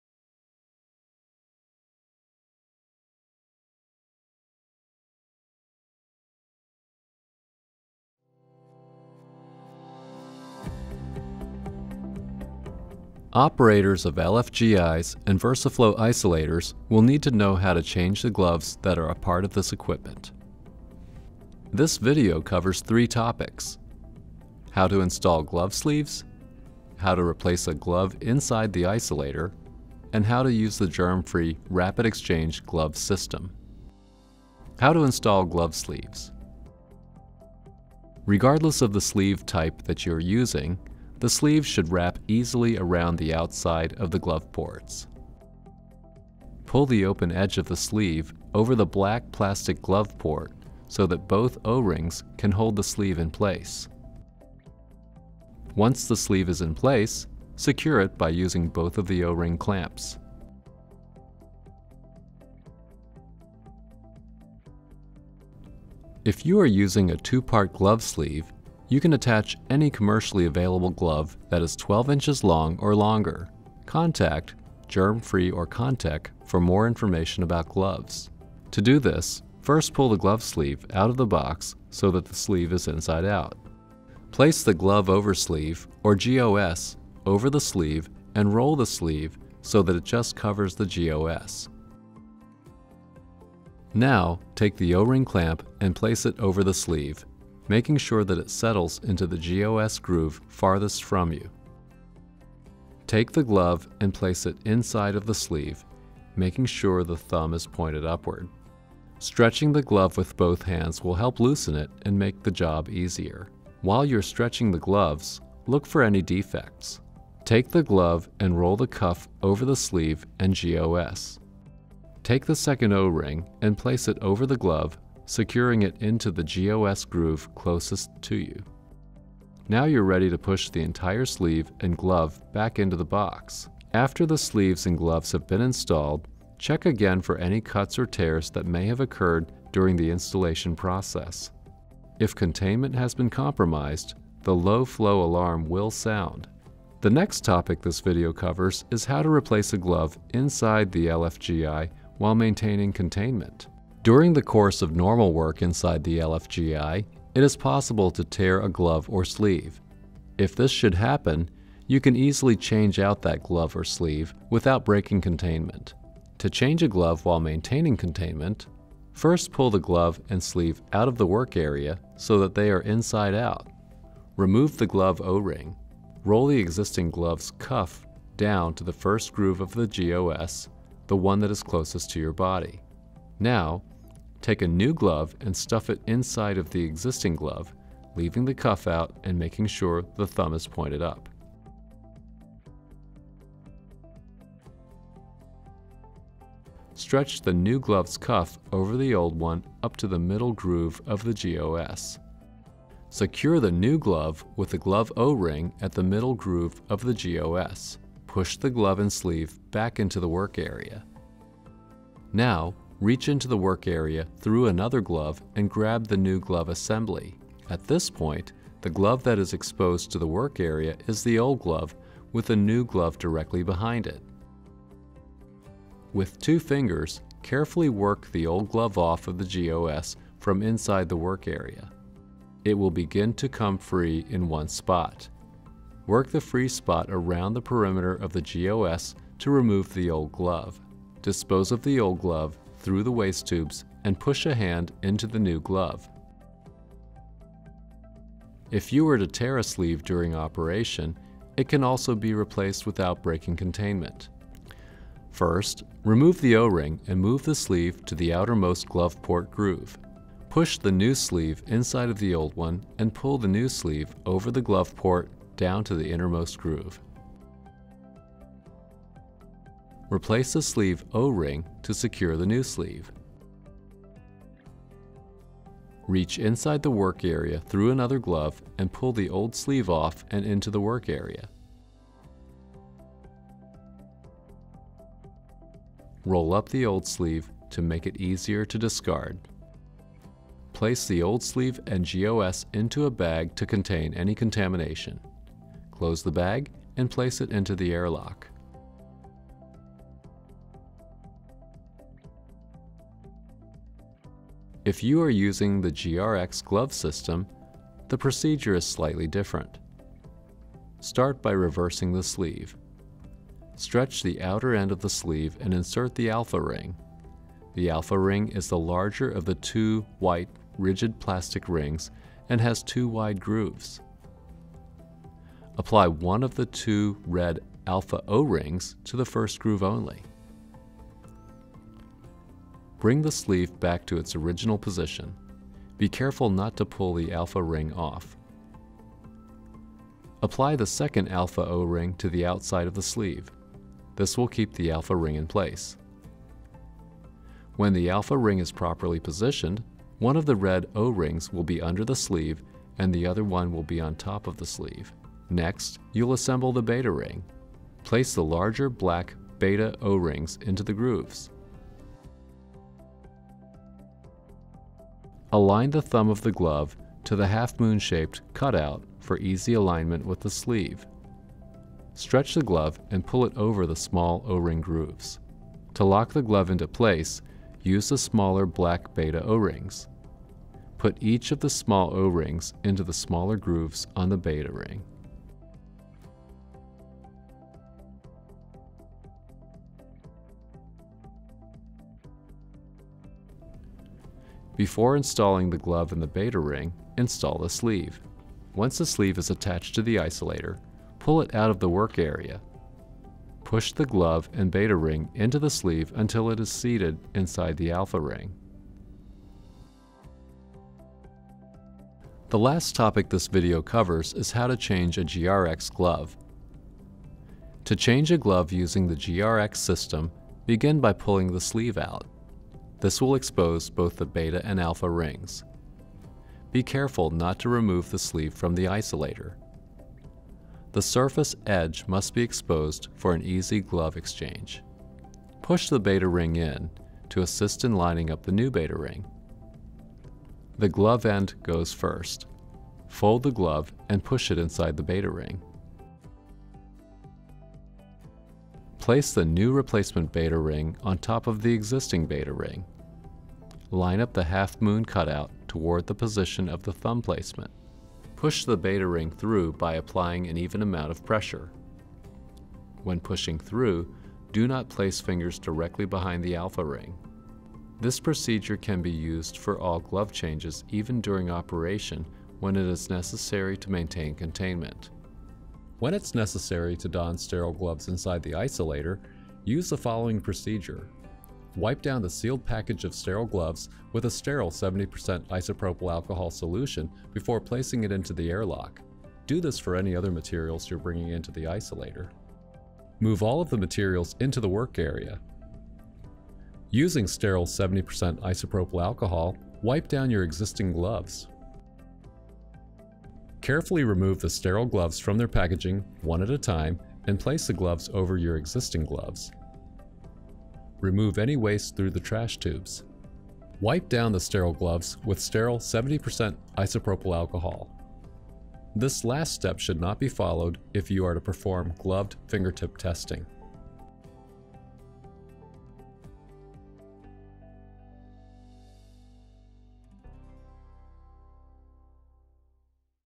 Germfree - Isolator - Glove and Sleeve Changes
Instruction for changing gloves and sleeves on Germfree's Compounding Aseptic Isolators and Compounding Aseptic Containment Isolators.  Includes information on changing gloves and sleeves while maintaining containment as well as the installation of sterile gloves and sleeves.